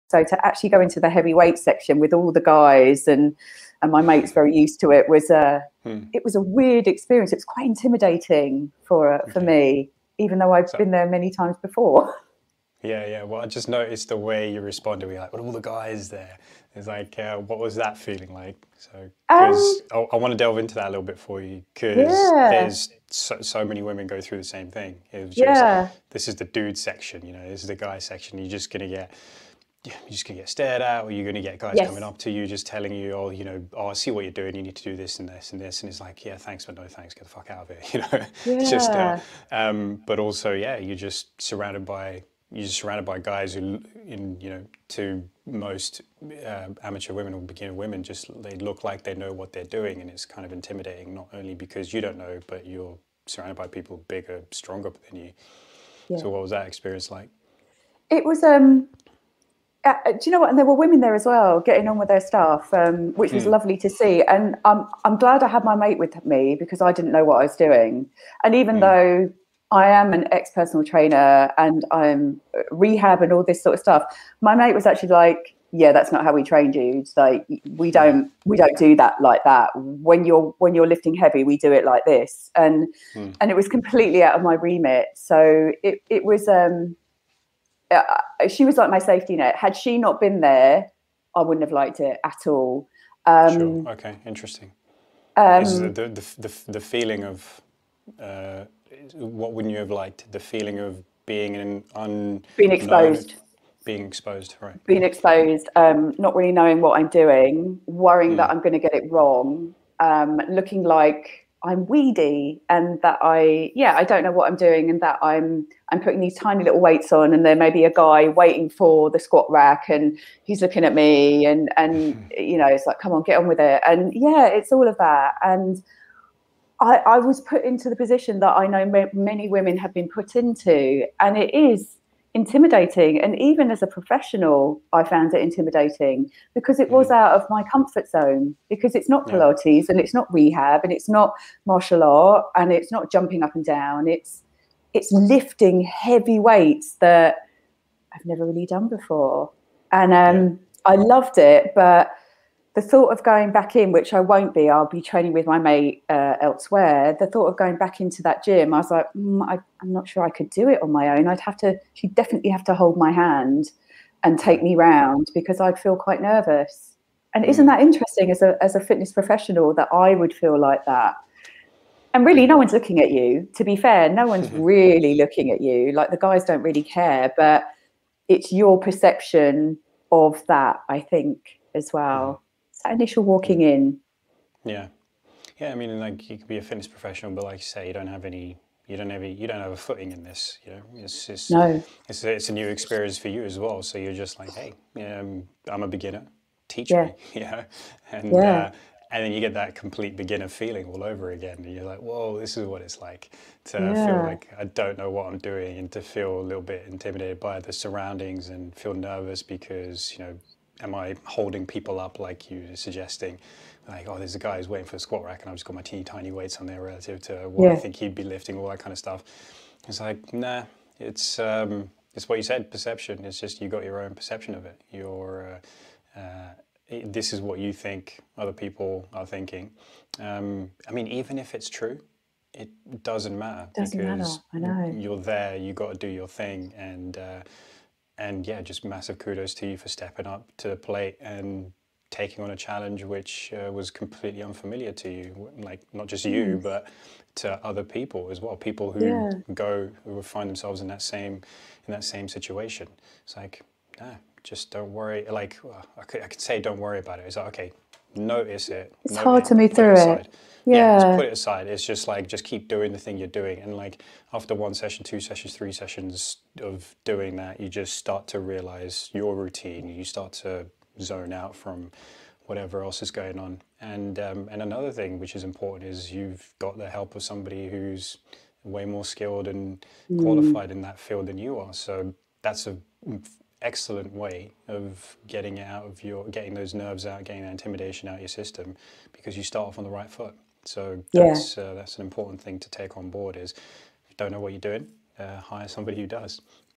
Feeling Intimidated In The Gym | Evolve Achieve THRIVE Clips
It's so common to experience feeling well out of your depth when you step onto the gym floor, especially for women.

How can you overcome that feeling? Watch the clip for a couple of suggestions...

or search 'Evolve. Achieve. THRIVE.' in your favourite podcast player.

If you like this then please let us know with a thumbs up and a comment.